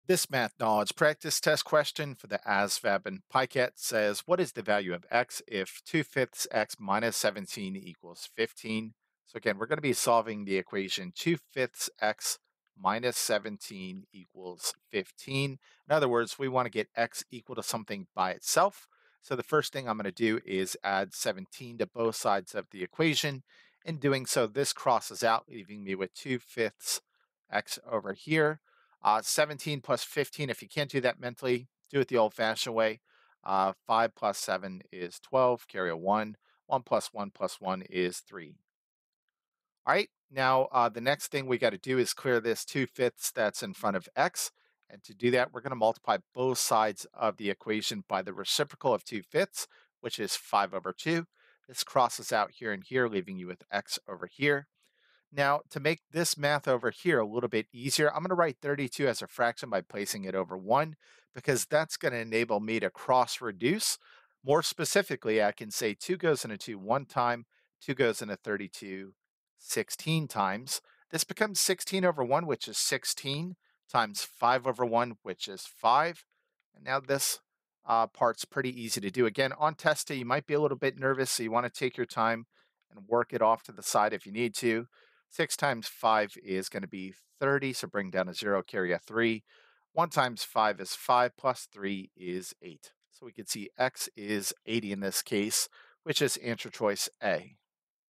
Solving Equations: ASVAB/PiCAT Math Knowledge Practice Test Question acetheasvab with grammarhero
​​​​​In this video, Grammar Hero works out a practice test question that requires you to solve an equation by clearing a fraction using a reciprocal. This question should closely mirror what you should expect to see in the Mathematics Knowledge (MK) subtest of  the  Armed Services Vocational Aptitude Battery (ASVAB) as well as the Pre-screening internet-delivered Computer Adaptive Test (PiCAT).